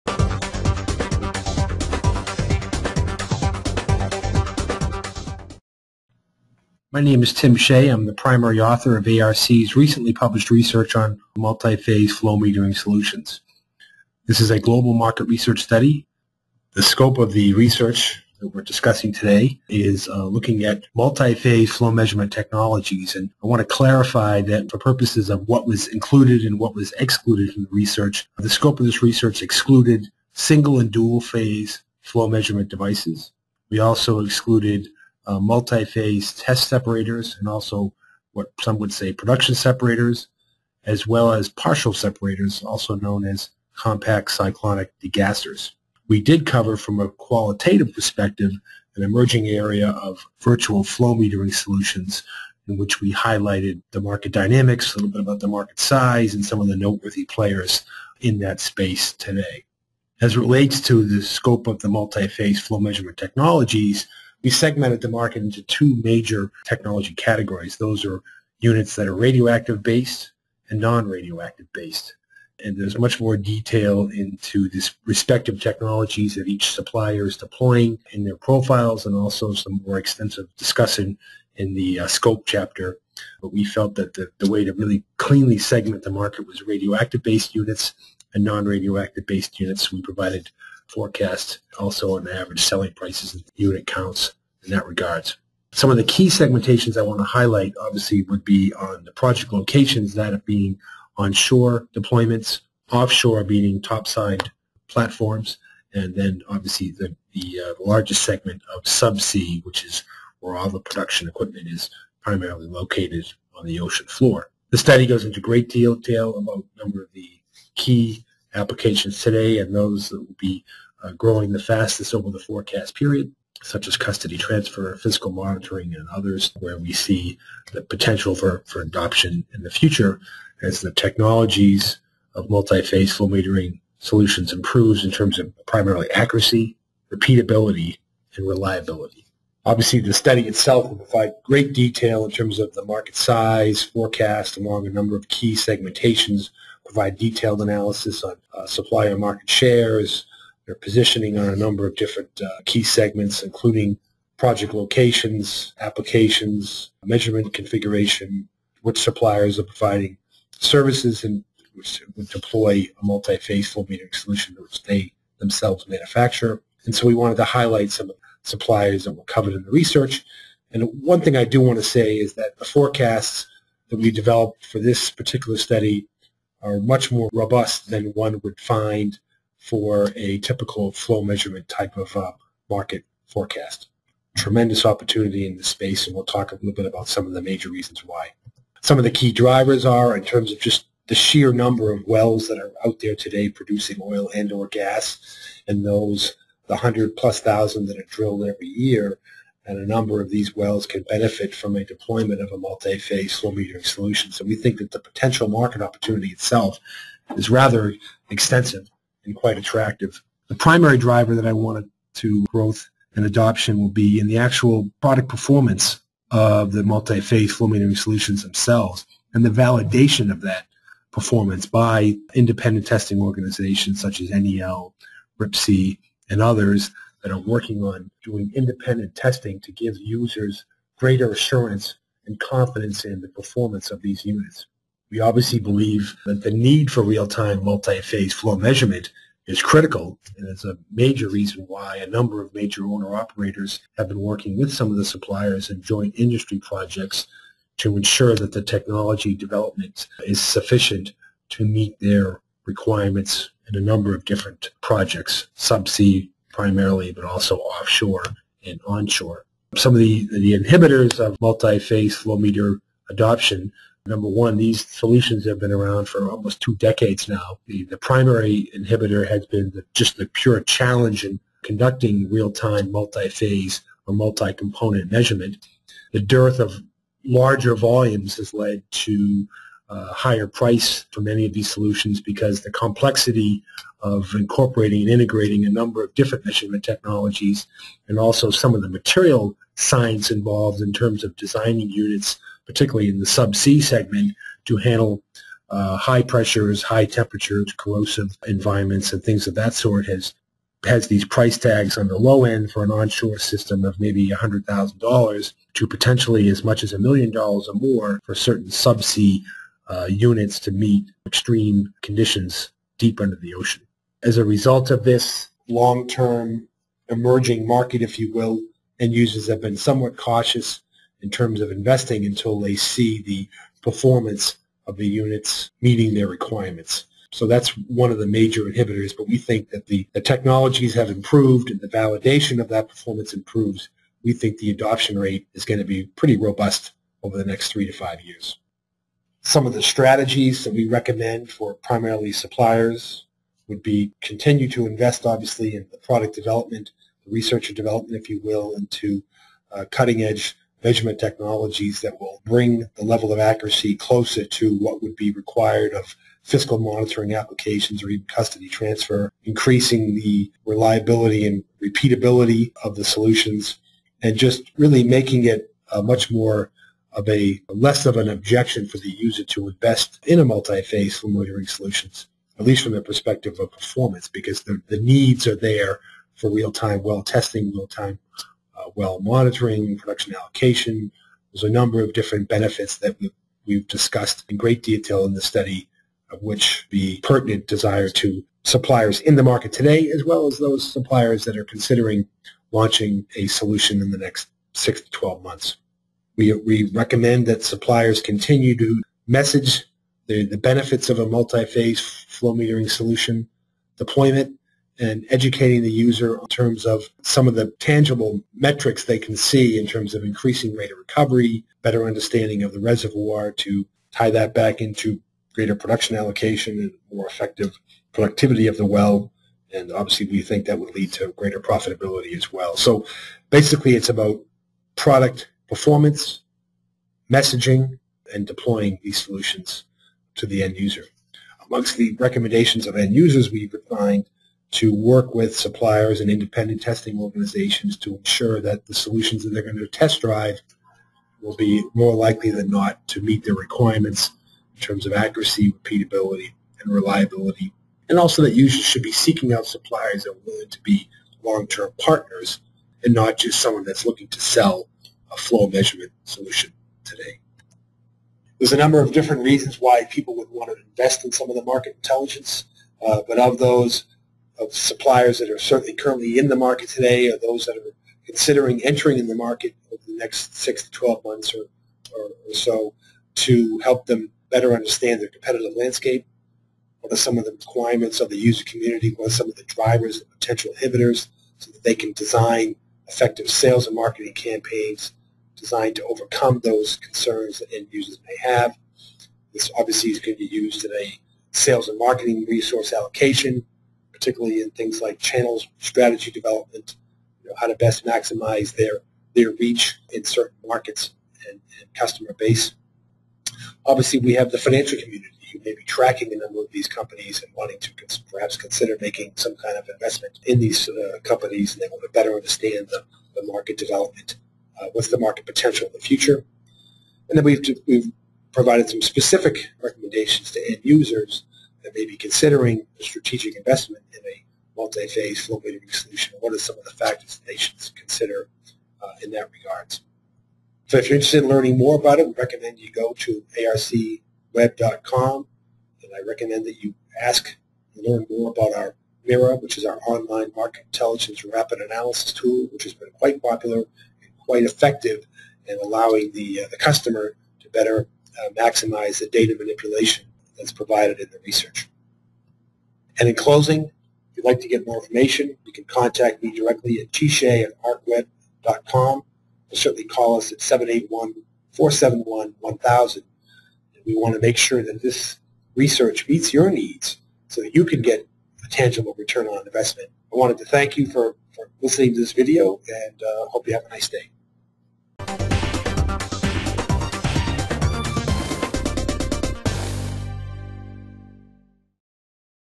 MultiPhase Flowmeter Global Research Study - Overview by Tim Shea, Analyst, ARC Advisory Group
Multi-Phase Flowmeters are covered in this comprehensive Global Market Research Study, by study author Tim Shea, ARC Analyst.  This study delivers a wide range of segmentations and includes market size, market shares, market growth, market forecasts, best practices and trends and strategies for success.